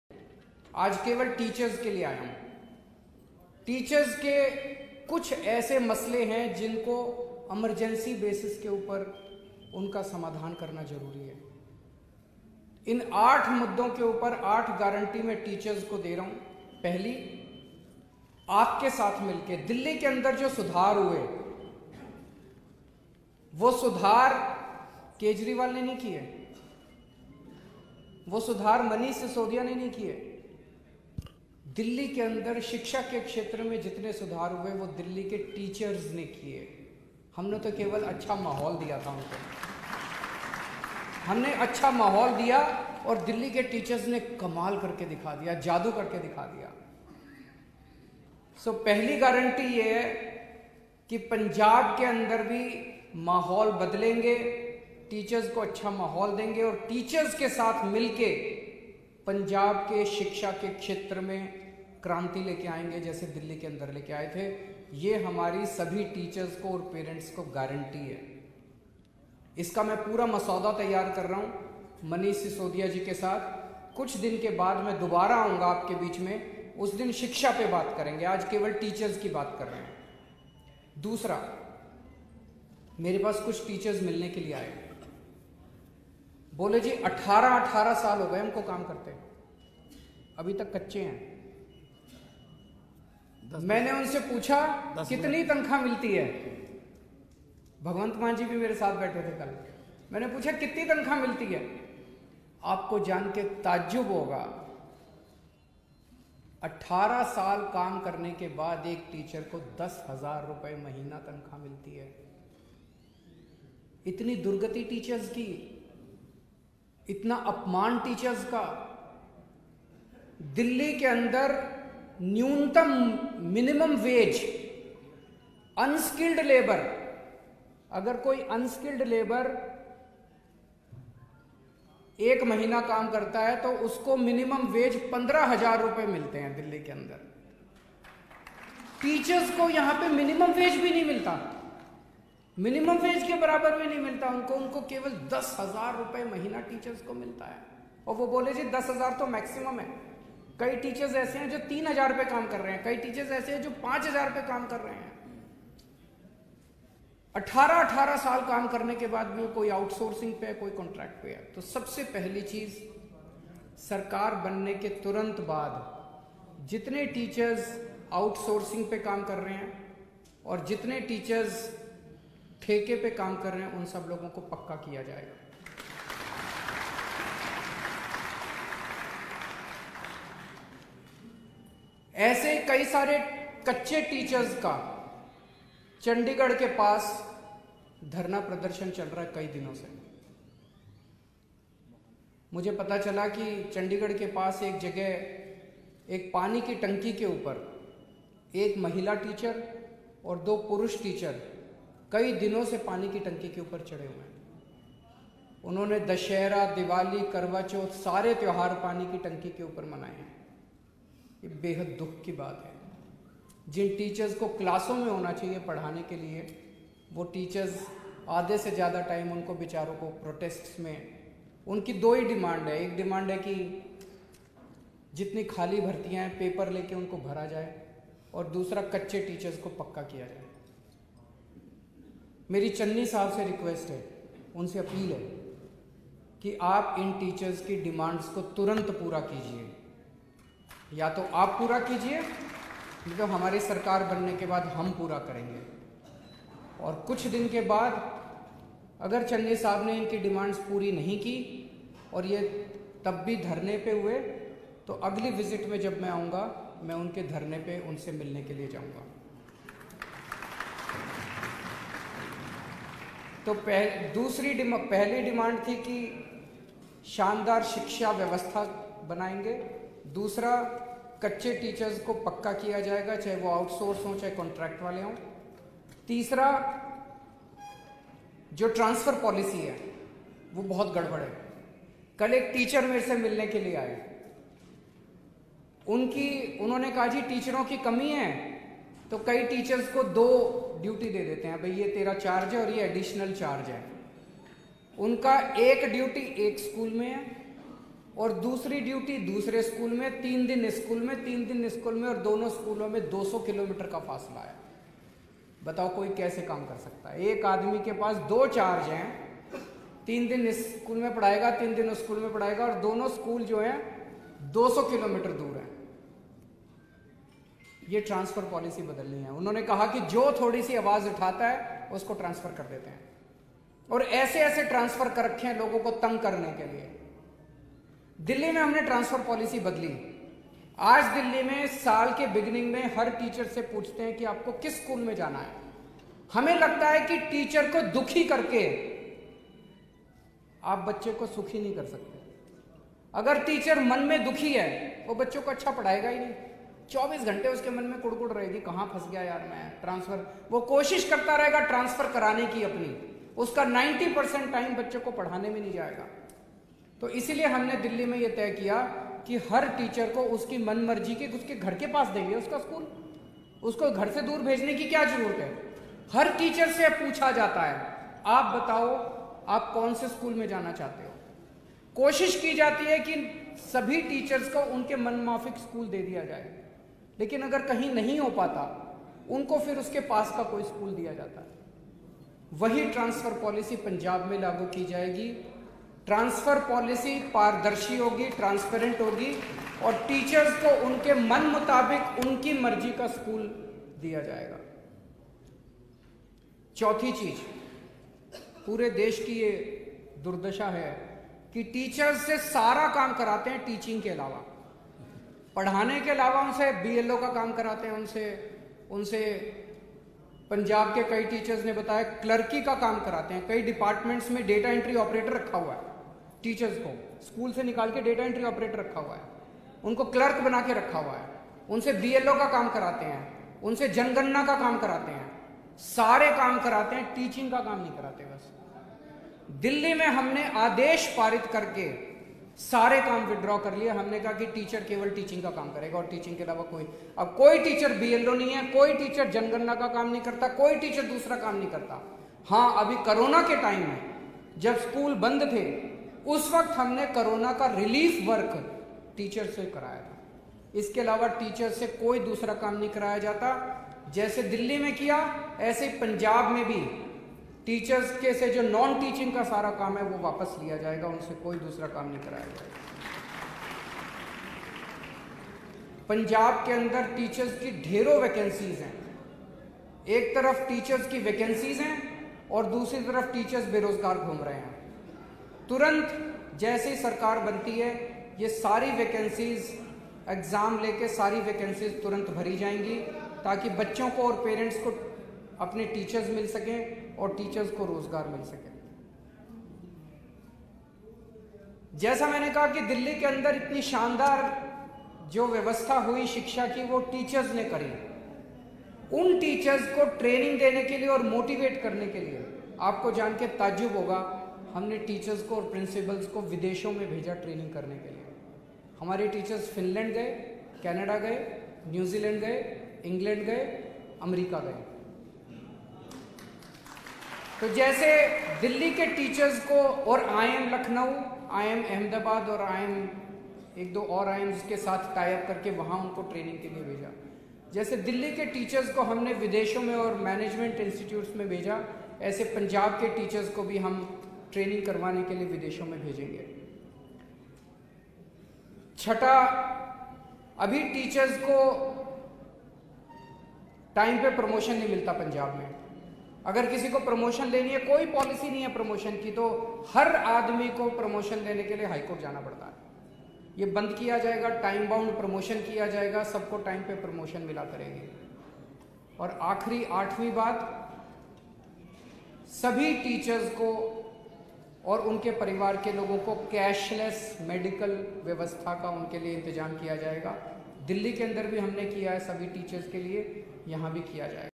ਟੀਚਰਾਂ ਦੀਆਂ ਸਾਰੀਆਂ ਪੋਸਟਾਂ ਭਰਾਂਗੇ, ਹਰ ਟੀਚਰ ਦੀ ਪੋਸਟਿੰਗ ਘਰ ਦੇ ਨੇੜੇ ਹੋਵੇਗੀ-ਕੇਜਰੀਵਾਲ | Punjabi Bulletin
Punjabi Bulletin